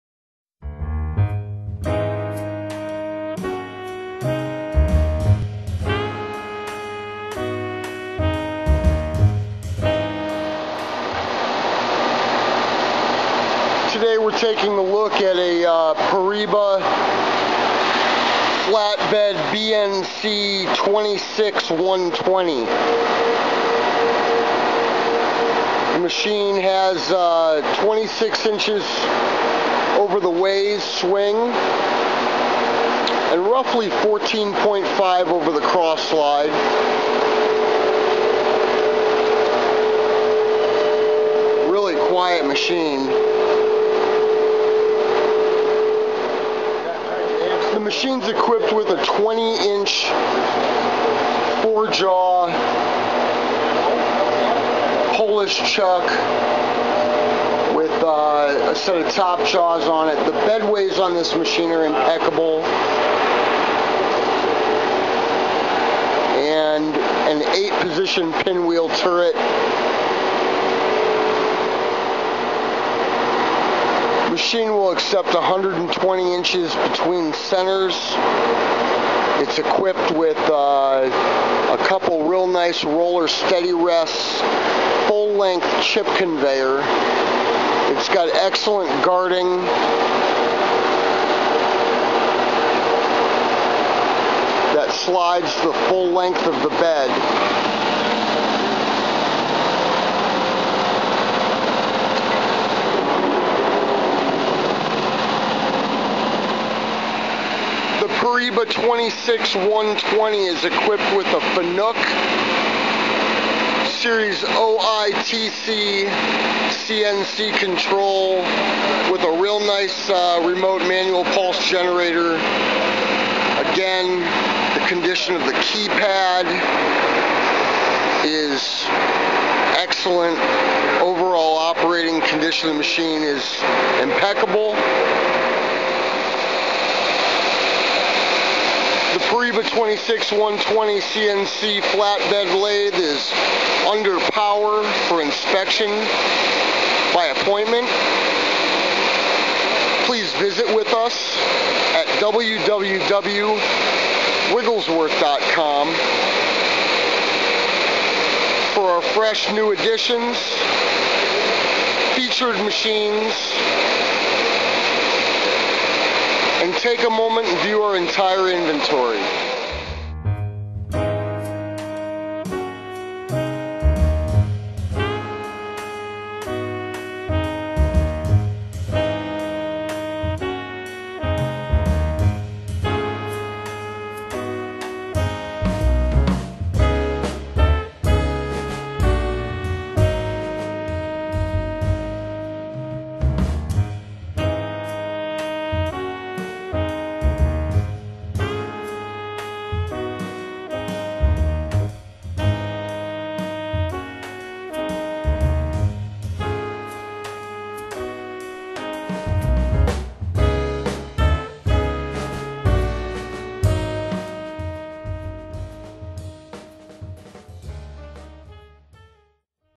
POREBA 26" X 120" CNC FLAT BED SHAFT LATHE
MODEL BNC 26120 X

YEAR 2007

 SPECIFICATIONS:

SWING 26"
SWING IN GAP 31"
 MAX. DISTANCE BETWEEN CENTERS 120"
MAX. TURNING LENGTH 118"
SWING OVER CROSS SLIDE. 16"
MAX. WORK PEICE WEIGHT. 4400 LBS.
SPINDLE SPEED RANGE. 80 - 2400 R.P.M.
SPINDLE BORE DIAMETER. 4.1"
SPINDLE NOSE TYPE A1-11
TURRRET MFG BARUFFALDI
NUMBER OF TURRET TOOLS 8
TOOL SHANK 1 1/2"
INDEXING TIME 0.42 SEC.
RAPID TRAVERSE RATE 585 I.P.M.
MAX. JOG RATE 118 I.P.M.
TAILSTOCK QUILL DIAMETER. 4"
TAILSTOCK QUILL STROKE. 6"
TAILSTOCK QUILL TAPER. NO. 5 M.T.
MAIN DRIVE MOTOR. 30 H.P.
 OVERALL DIMENSIONS:
LENGTH 204"
WIDTH 87"
HEIGHT 80"

 FEATURES & EQUIPPMENT:

 FANUC OiTC CNC CONTROL
 20" 4-JAW CHUCK W/ 4.1" HOLE
 FULL LENGTH REAR CHIP GAURDING
 COOLANT SYSTEM W/ PULL OUT PANS & PUMP
 FULL LENGTH CHIP CONVEYOR
 MOBIL OPPERATOR CHIP GAURDING W/ WINDOW
 2) STEADY RESTS

 UNDER POWER FOR INSPECTION - SEE VIDEO

 SPECIFICATIONS SUBJECT TO VERIFICATION

T.R. Wigglesworth Machinery Company
7100B Krick Road